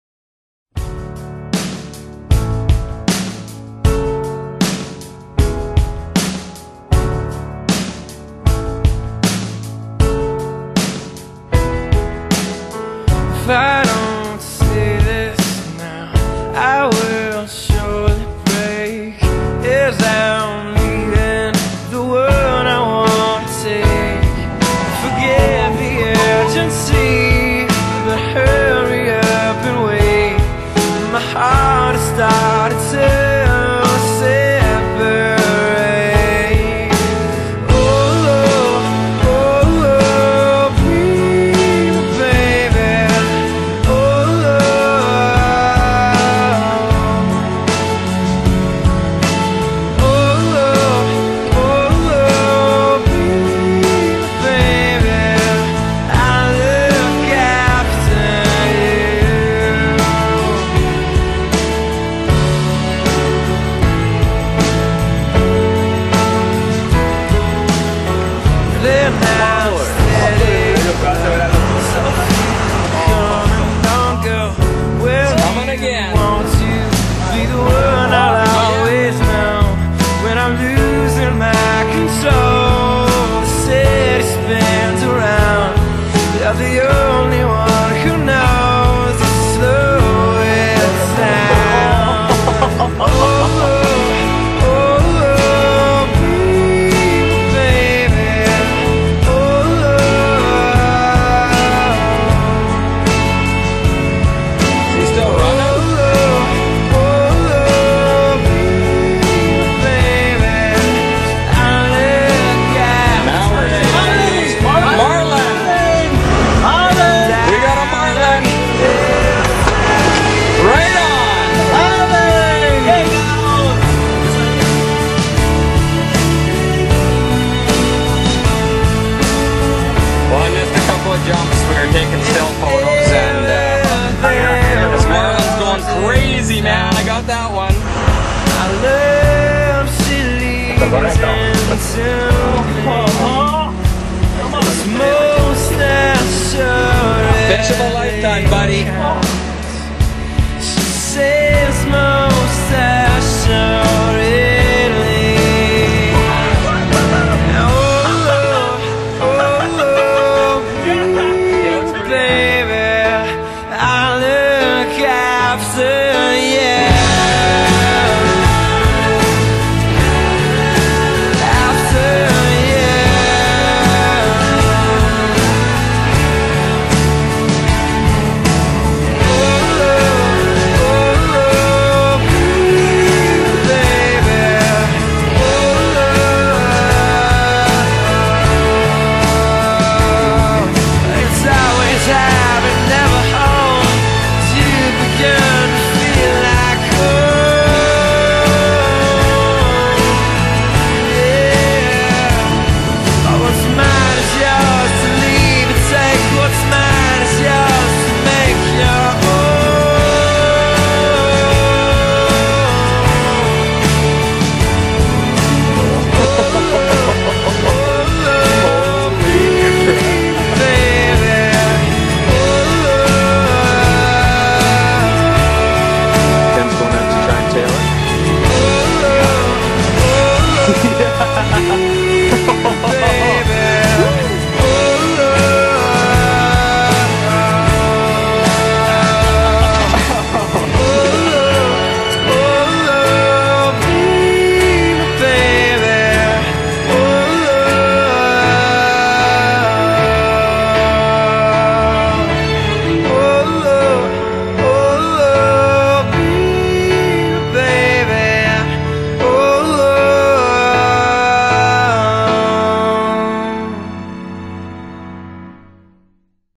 BIG GAME FISH
Fishing from around the world this past year